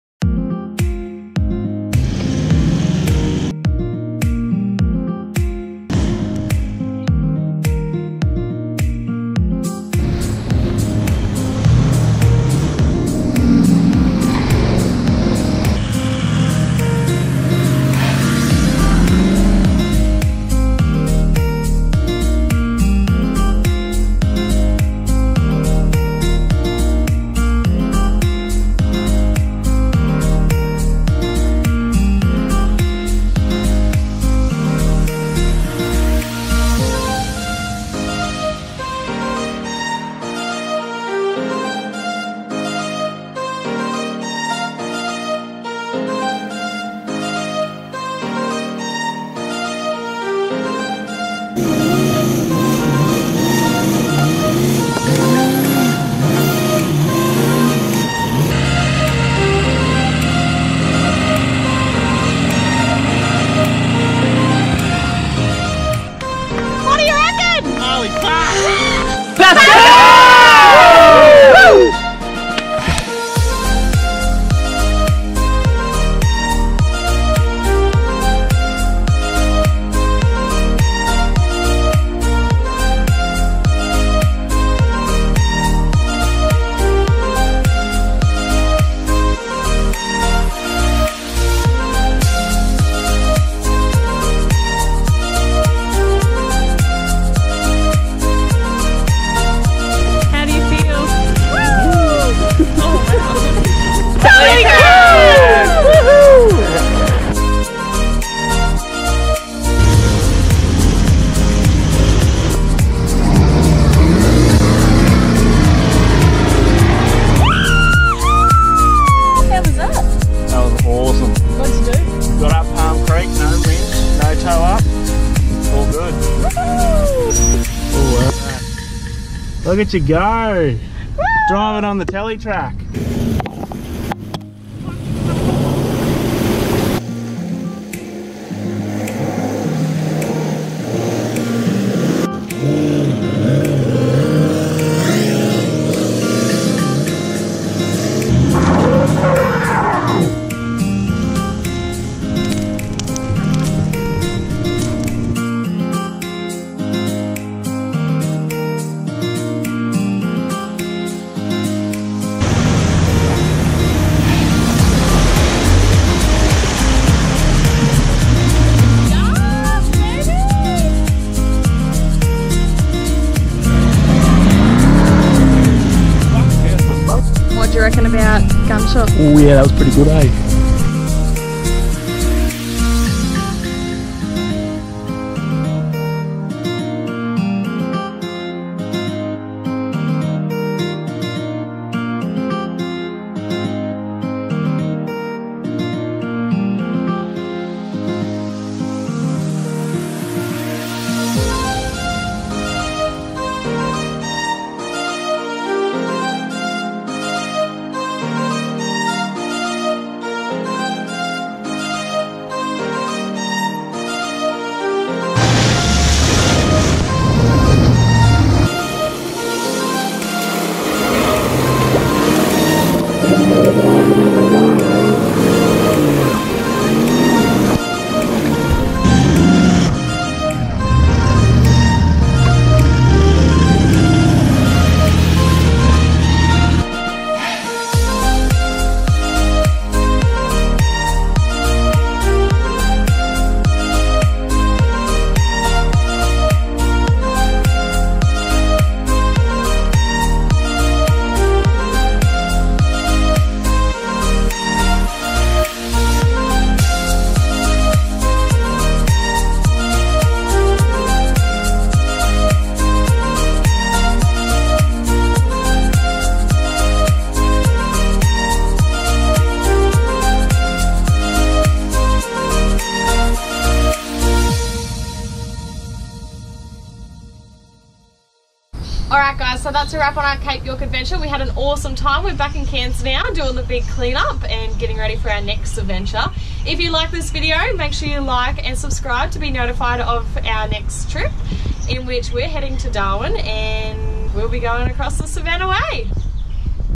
Cape York Part II
Come cruise with us for the top end of Cape York Peninsula in which we tow our Jayco Silverline all the way to the tip! We travel from Weipa complete two 4WD tracks (Frenchman's & Old Telegraph Track), manage to get the tip to ourselves for Sunset & enjoyed a Beer each.